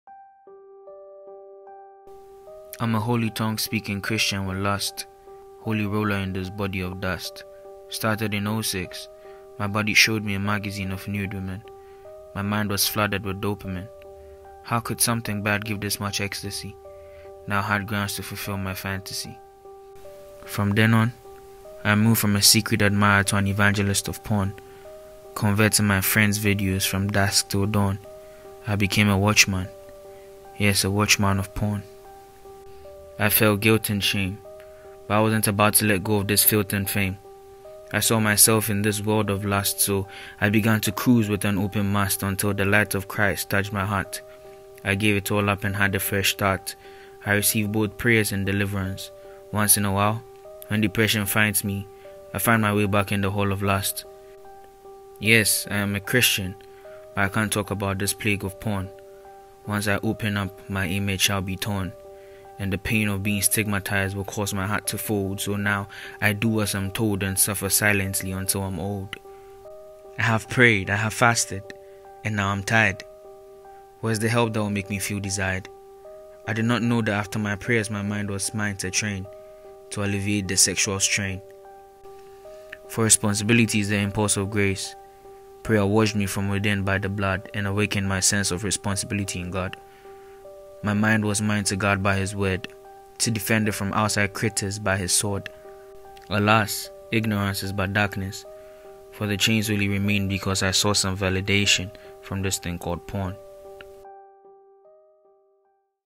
A Christian with Porn
The truth that is hardly spoken of, the prison without walls that men are bound to, but fail to flee to their freedom, the cancer which eats you up from within till you are left with nothing.
Yet one thing stands, there is sufficient grace and responsibility is the impulse of grace!